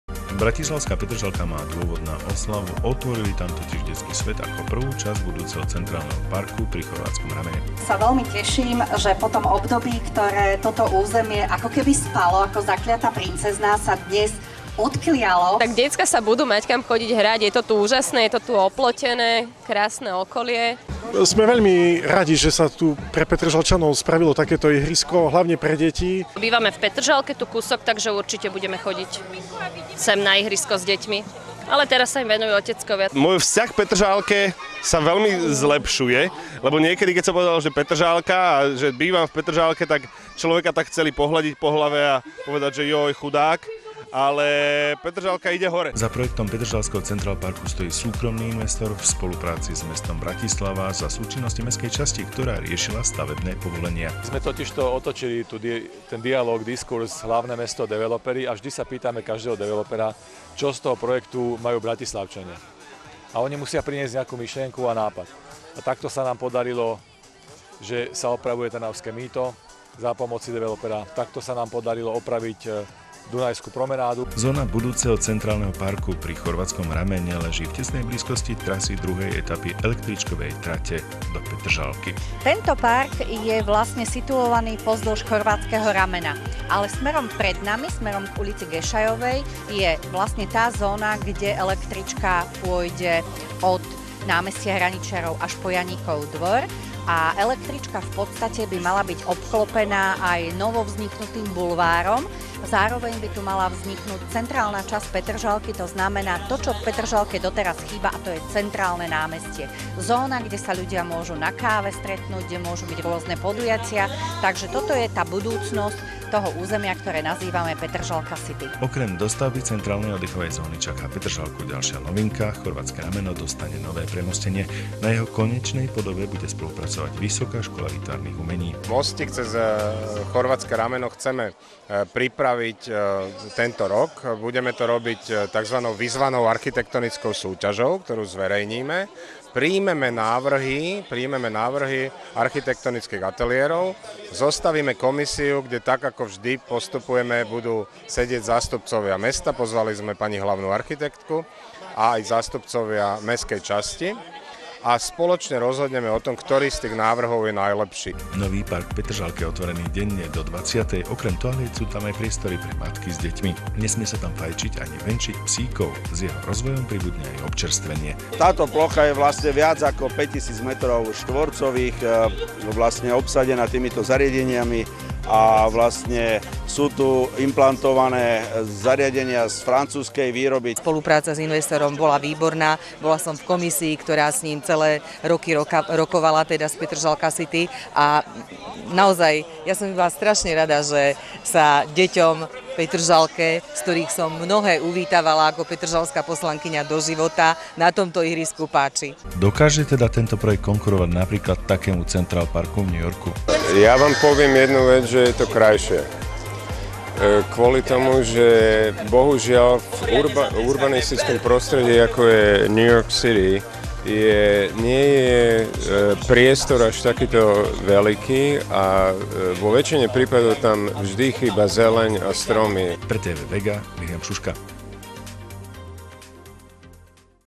Vega Tv - Central park v Petržalke
Otvorili jeho prvú časť - Detský svet.